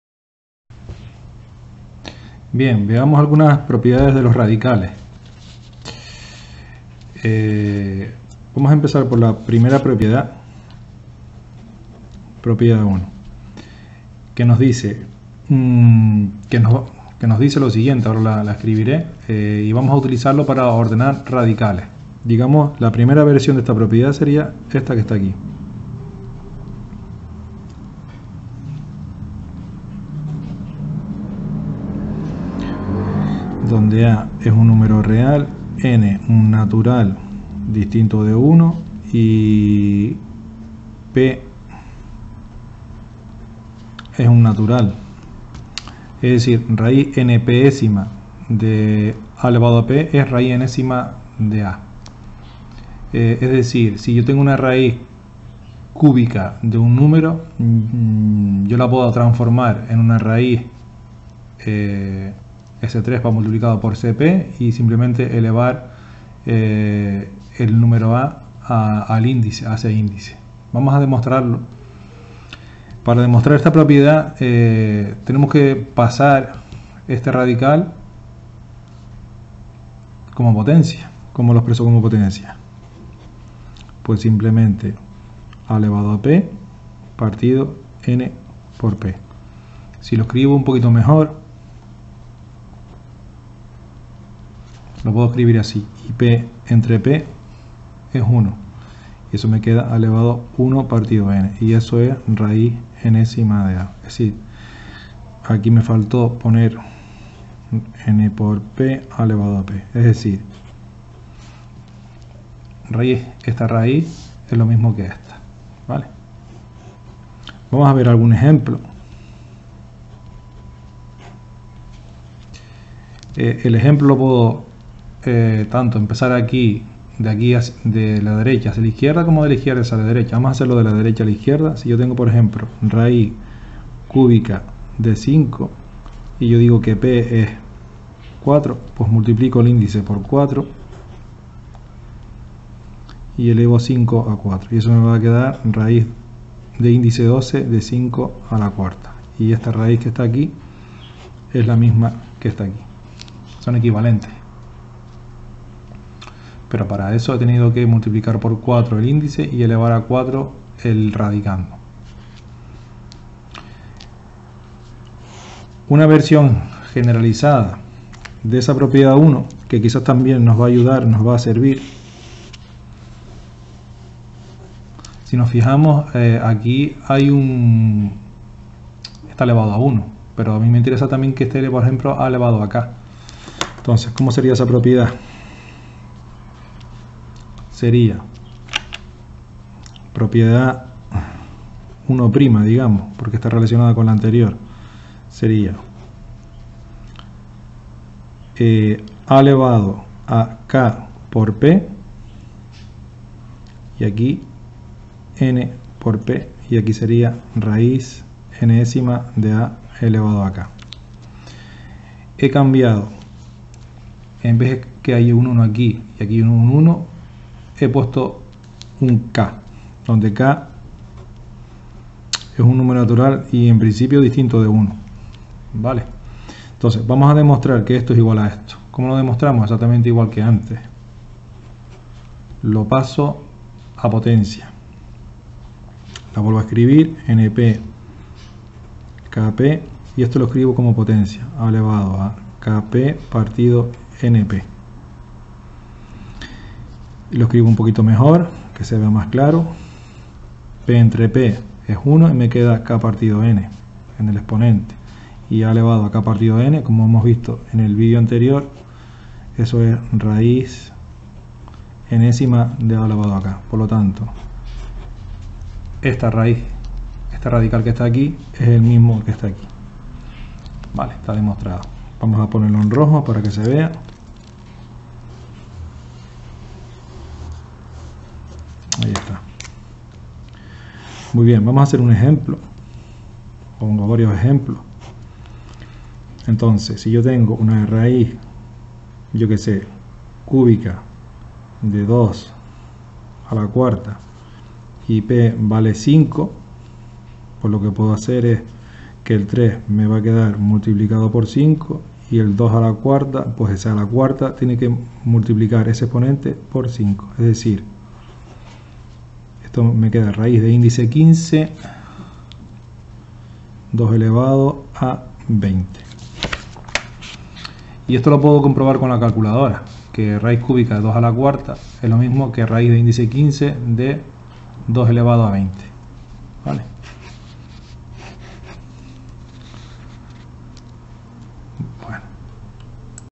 Propiedad 1 Radicales
Se define y demuestra la propiedad 1 de Radicales.